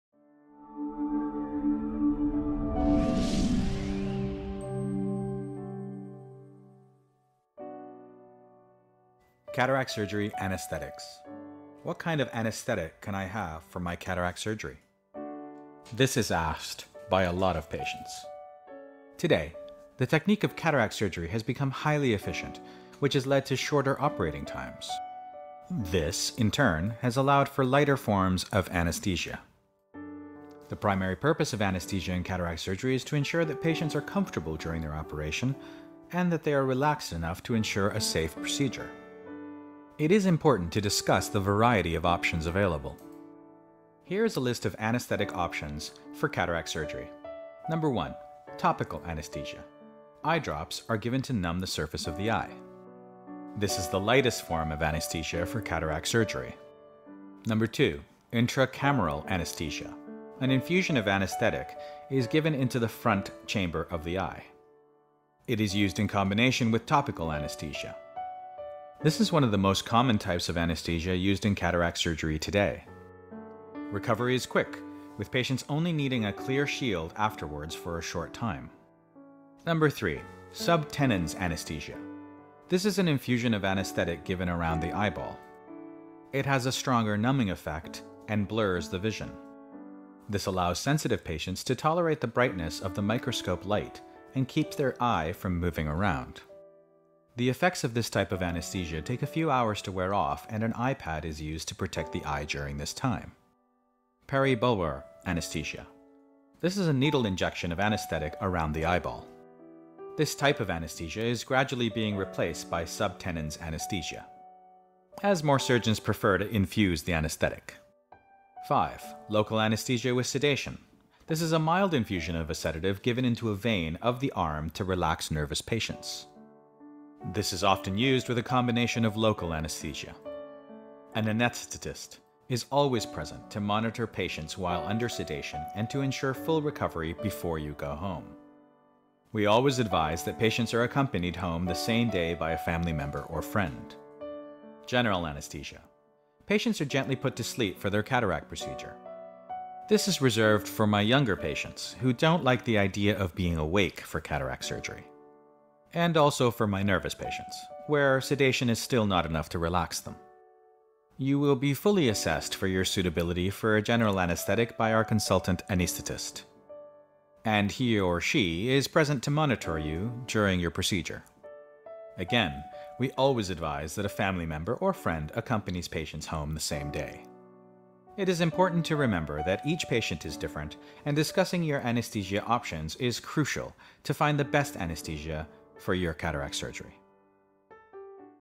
Cataract surgery Anaesthetics
Cataract surgeons (ophthalmologists) perform cataract surgery over 300,000 times per year in the UK alone (according to the NHS).